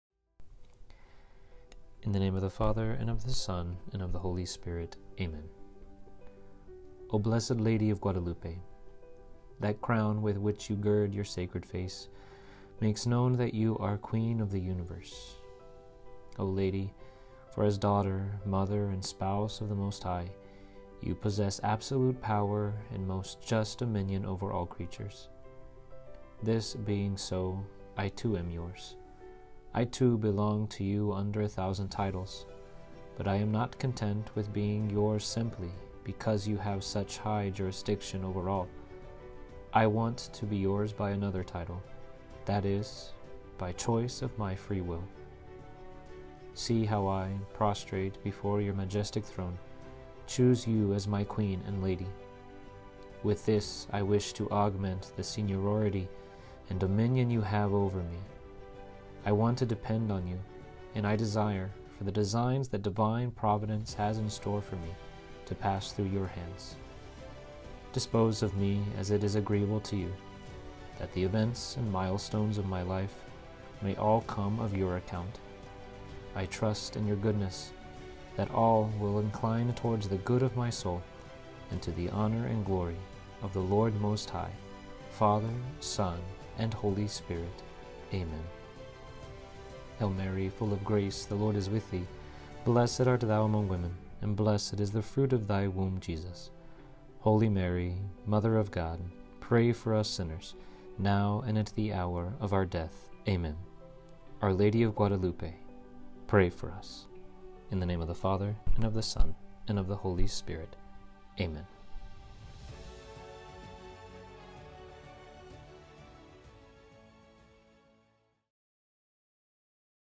Guadalupe Novena: Day One
Day One of the novena in honor of Our Lady of Guadalupe. Here is the prayer for today:
Oh Blessed Lady of Guadalupe! That crown with which you gird your sacred face makes known that you are Queen of the Universe. O Lady, for as Daughter, Mother, and Spouse of the Most High you possess absolute power and most just dominion over all creatures.

This being so, I too am yours; I too belong to you under a thousand titles, but I am not content with being yours simply because you have such high jurisdiction over all; I want to be yours by another title, that is, by choice of my free will.

See how I, prostrate before your majestic throne, choose you as my Queen and Lady. With this I wish to augment the seniority and dominion you have over me; I want to depend on you and I desire for the designs that Divine Providence has in store for me to pass through your hands.

Dispose of me as it is agreeable to you, that the events and milestones of my life may all come of your account. I trust in your goodness, that all will incline towards the good of my soul and to the honor and glory of the Lord Most High, Father, Son and Holy Spirit. Amen